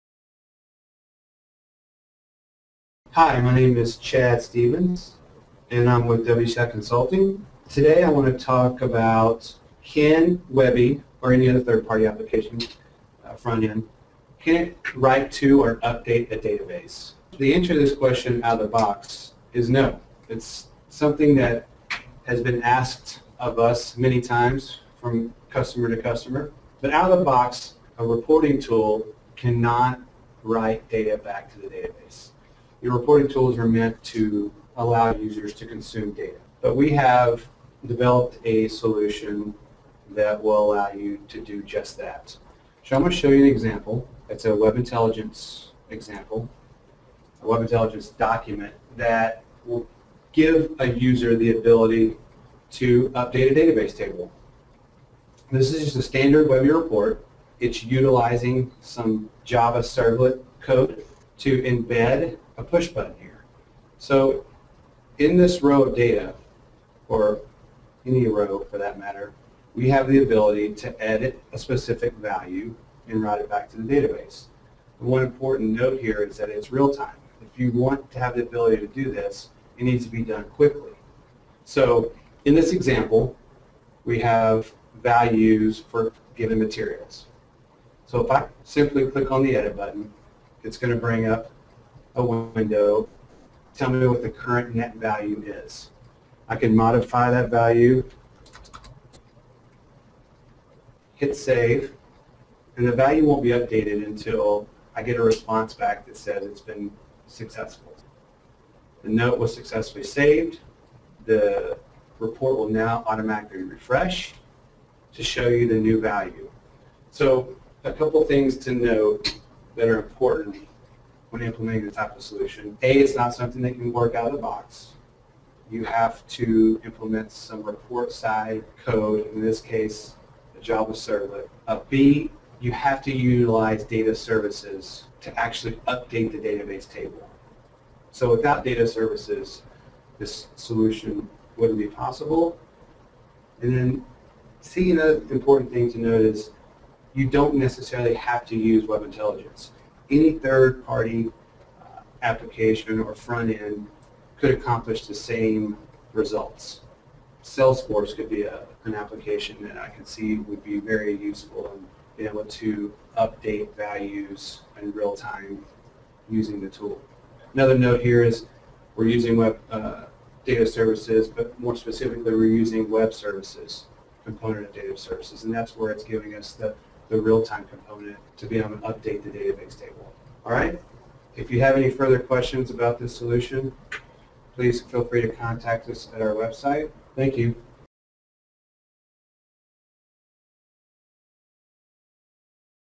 Can you Update a Database with Webi? SAP BusinessObjects Web Intelligence Tutorial
In this video we explore if SAP BuinessObjects Web Intelligence (Webi), or any other 3rd party front end application, can write an update to a database.

Spoiler alert...out of the box it can't! But watch how we figure out how to edit a database in real-time using Webi.